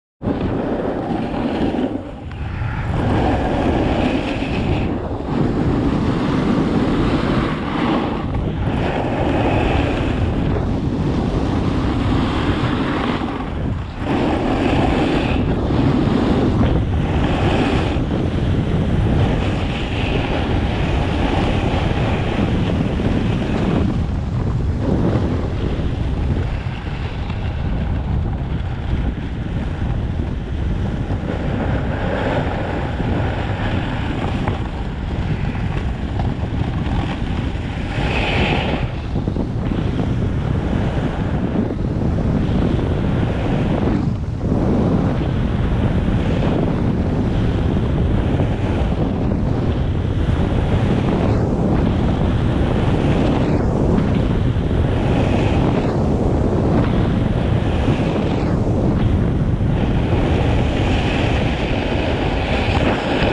2月 雫石ゲレンデ ジャイアントスラローム後編
ゲレンデ状態もまずまず。
空いてるし、雫石は隠れた名ゲレンデだと思う(●´ω｀●)